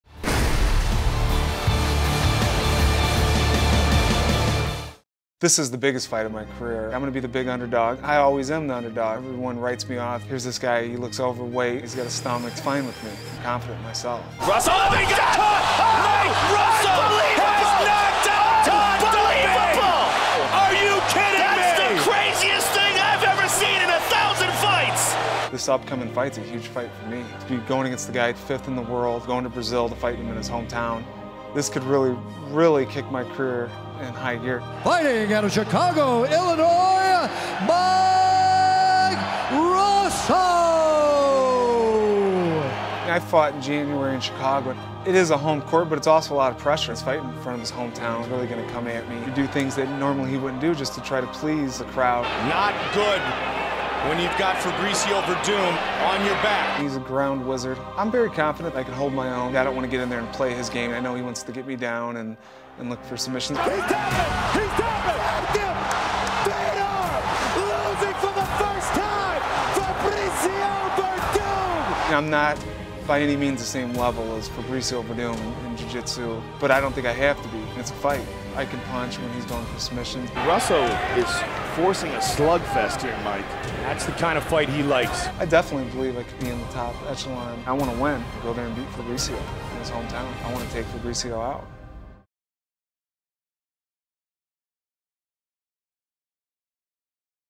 UFC 147: Mike Russow Pre-fight Interview
"I want to take Fabricio out." Mike Russow is happy to be the underdog against Fabricio Werdum. Hear why the Chicago native feels all the pressure is on his opponent at UFC® 147.